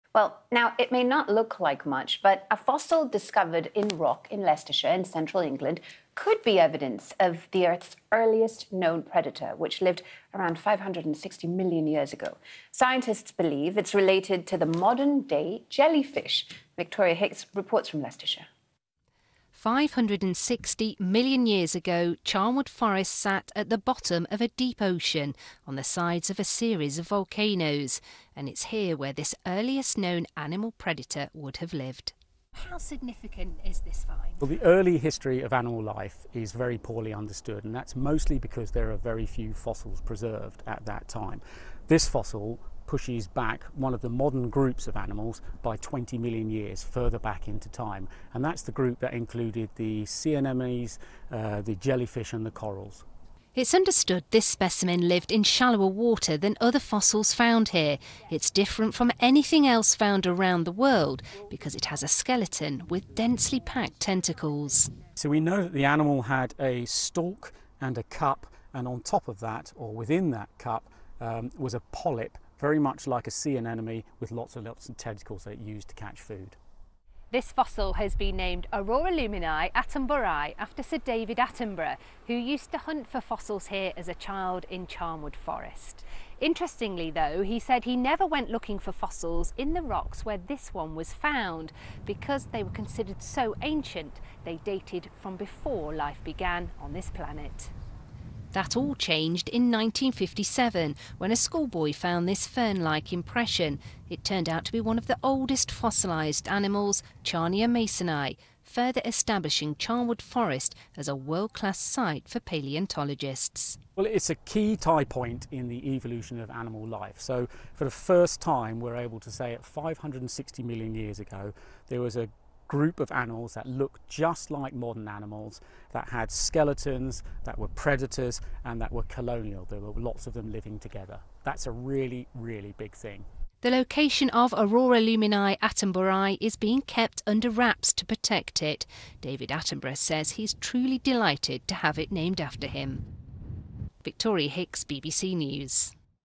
Fossil of ‘earliest animal predator’ is named after David Attenborough (UK)
Sea creature, thought to have used tentacles to capture food, is named Auroralumina attenboroughii.
Discovered in Charnwood Forest, Leicestershire (England).